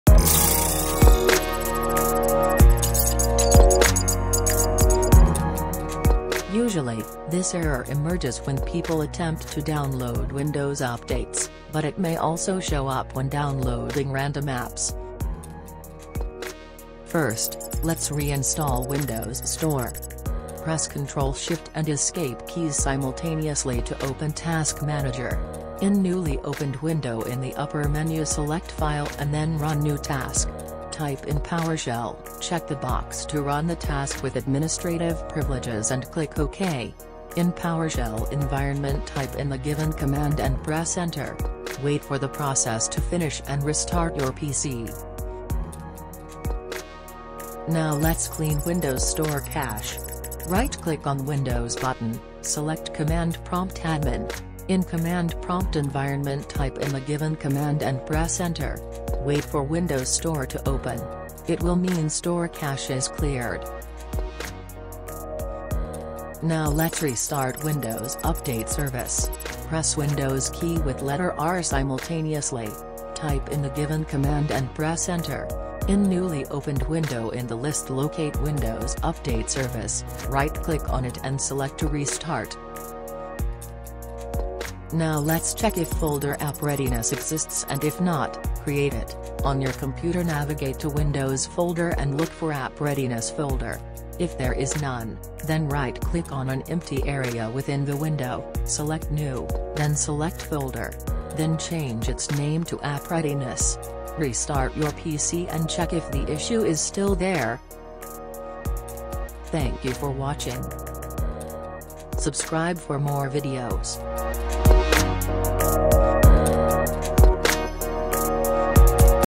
How to Fix Windows Store Error Code: 0x80073cf9
Windows Store Error Code: 0x80073cf9 usually appears when user updates Windows or try installing new app from Windows Store. We provide users with a detailed fixing tutorial which can be found

If 0x80073cf9 error is related to Windows store it essentially prevents users from installing apps and it will provide users with two options. Either to try again with the installation or to cancel it. Neither of them helps with the issue.

Method 1: 00:15
Method 2: 00:47
Method 3: 01:05
Method 4: 01:24